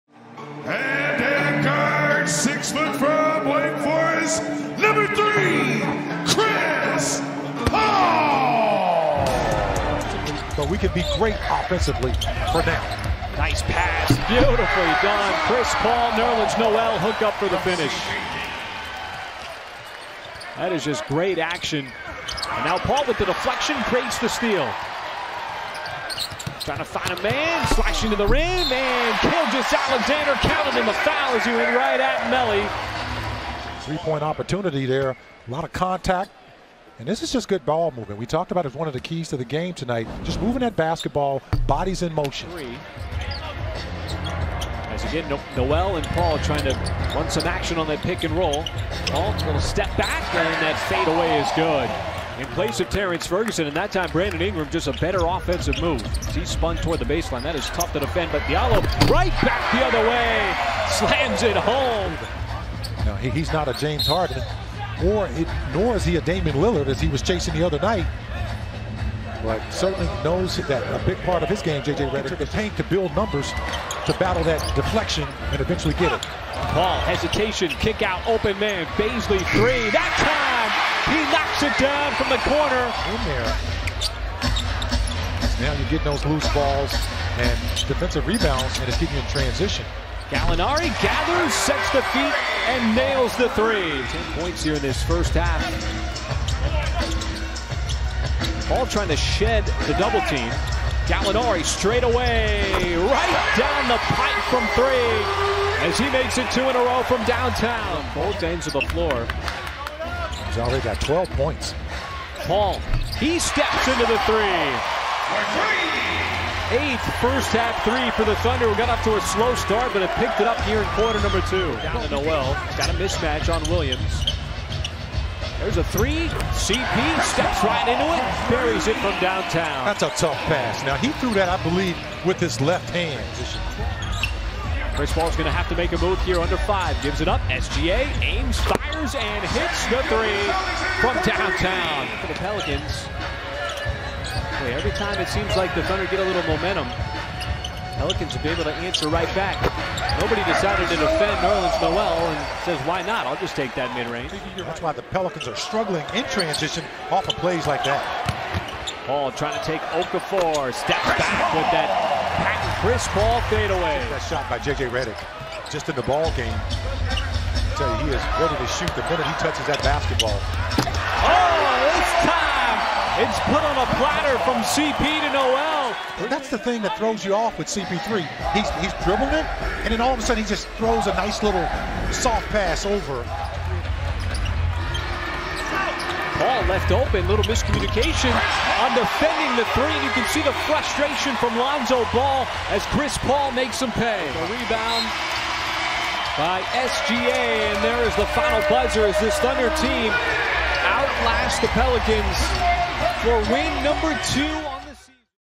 Chris Paul's Full Highlights: 15 PTS, 9 AST vs Pelicans | 2019-20 NBA Season
• 15 points (5-9 FG, 3-5 3P, 2-2 FT)
• 1 rebounds
• 9 assists
• 3 steals, 0 blocks, 3 turnovers, 2 fouls
• 11 +/-
• 29 minutes